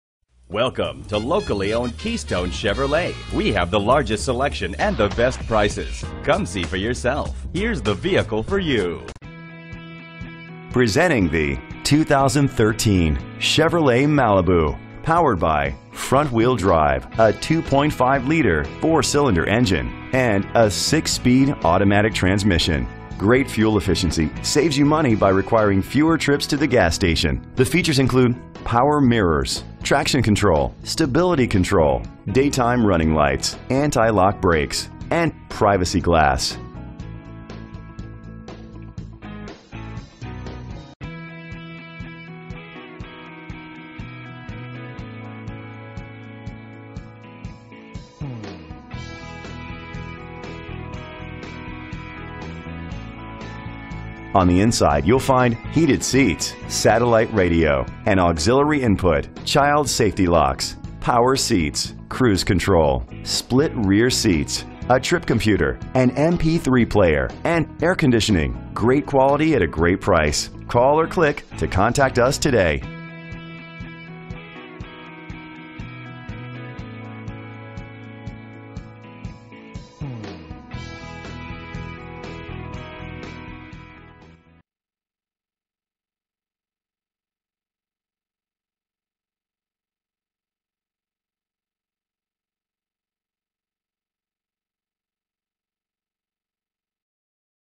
2013 Chevrolet Malibu - Sand Springs OK
Year: 2013
Make: Chevrolet
Model: Malibu
Trim: LT
Engine: 2.5 liter 4 cylinder FWD
Transmission: 6-Speed Automatic
Color:
Mileage: 0
Address:
8700 Charles Page Blvd